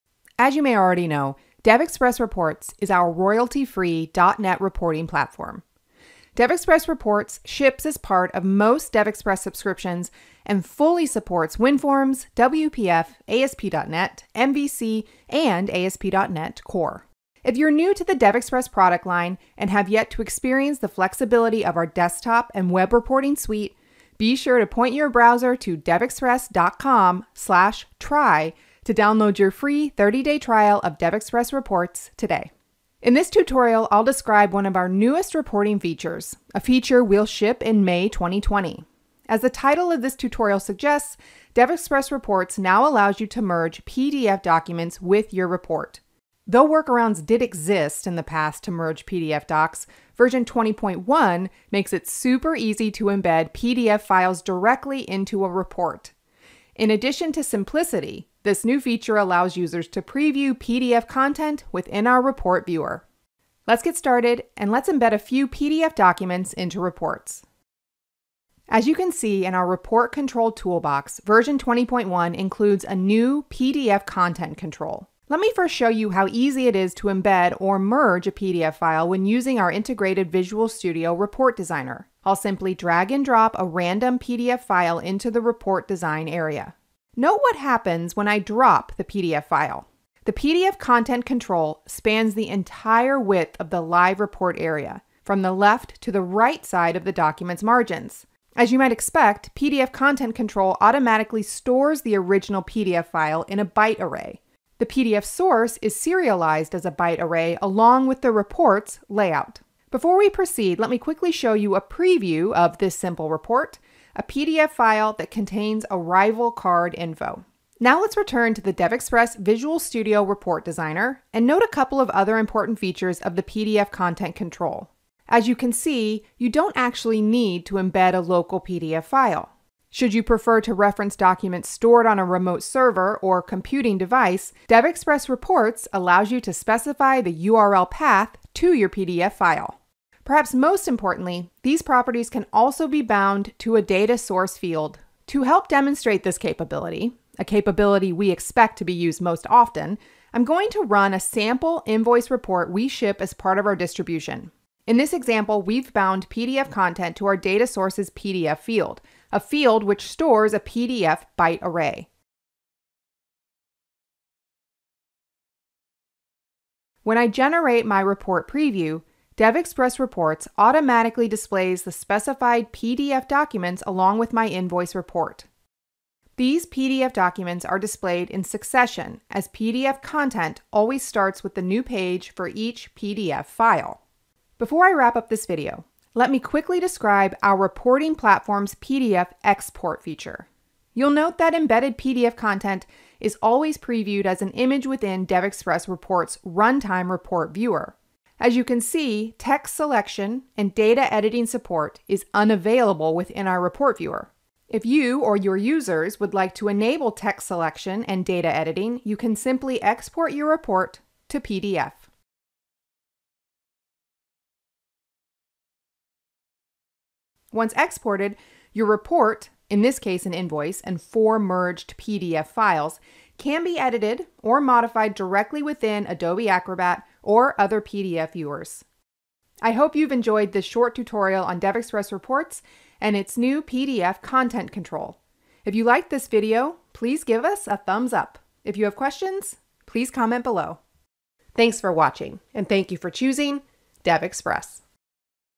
DevExpress Reports: Embedded PDF Documents
In this tutorial, we’ll describe one of our newest reporting features – a feature we will ship in May 2020.  As the title of this tutorial suggests, DevExpress Reports now allows you to merge PDF documents with your report. Though workarounds did exist in the past to merge PDF docs, v20.1 makes it super easy to embed PDF files directly into a report. In addition to simplicity, this new feature allows users to preview PDF content within our report viewer.